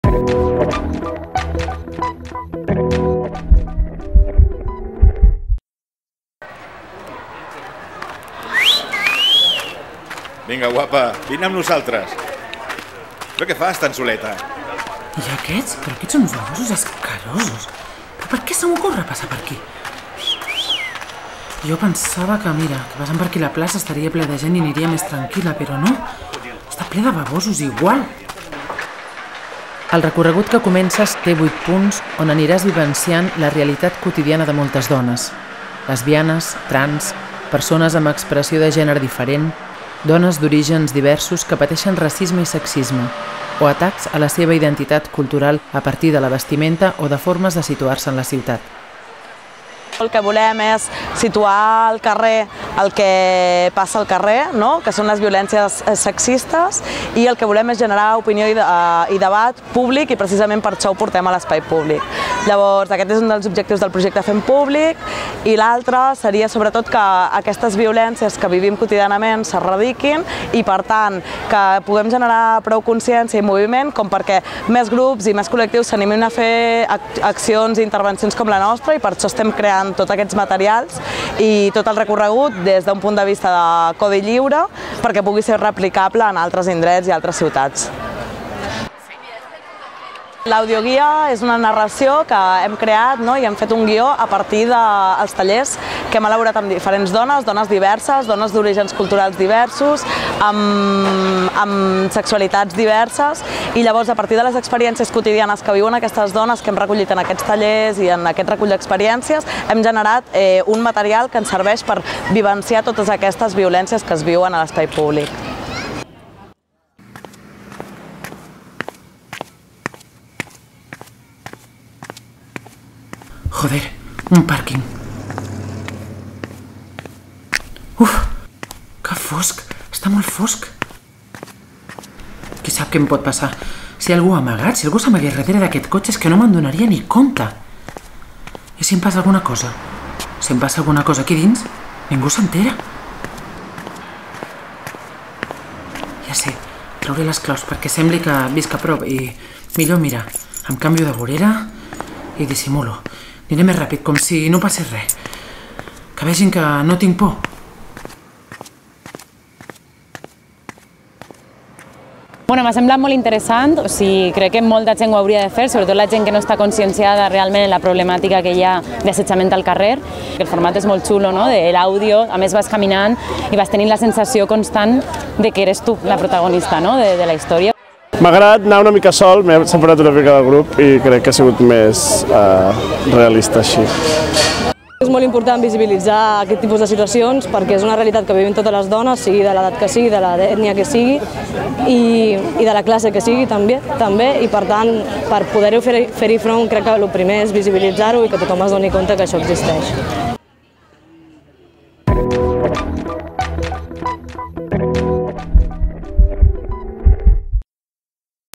Una ruta audioguiada reprodueix les violències que pateixen al carrer quotidianament les dones
Redacció i Audiovisuals: Sònia Calvó

"Guapa! Vine amb nosaltres! Què fas tan soleta?". Aquest és només un fragment d'un dels vuit àudios que s'han pogut escoltar a la ruta FemPúblic, una audioguia que vol mostrar als participants la realitat i les violències que pateixen al carrer quotidianament les dones. L'acció va tenir lloc el passat divendres, 25N, amb motiu del dia internacional per l'eliminació de les violències contra les dones.

Es tracta d'una iniciativa impulsada per Irídia Centre per la Defensa dels Drets Humans, la Cooperativa de Tècniques i el col·lectiu d'urbanisme amb perspectiva de gènere Punt6. "Volem situar al carrer el que passa al carrer, les violències sexistes, i generar opinió i debat públic" afirma Carla Alsina, coordinadora de l'àrea de gènere d'Irídia. Per elaborar-la s'ha treballat amb diferents dones, d'orígens i sexualitats diverses, a partir de les experiències recollides en diversos tallers. L'audioguia també vol oferir recursos per poder tornar a repetir l'experiència a altres llocs.